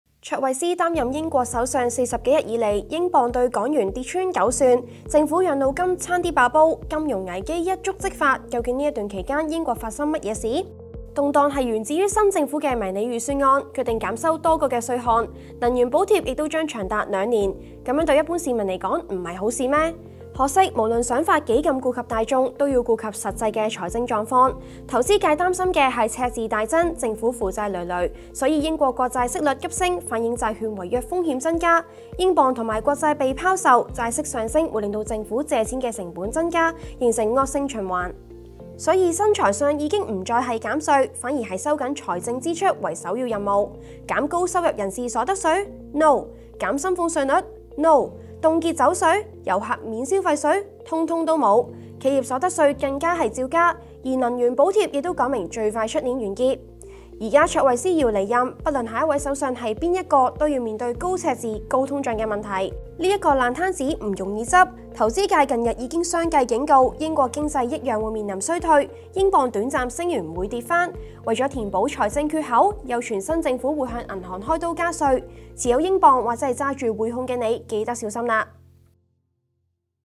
英國到底發生過什麼事 1分鐘睇清 點解減稅會令首相下台 英鎊仲會繼續升嗎？
很久不見　到底發生過什麼事？卓慧思擔任英國首相40幾日以來，英鎊兌港元跌穿9算，政府養老金差啲爆煲。講到未來，只有懷疑與懷疑! 春分秋至，到底卓慧思經過什麼事？我地用1分鐘睇清楚!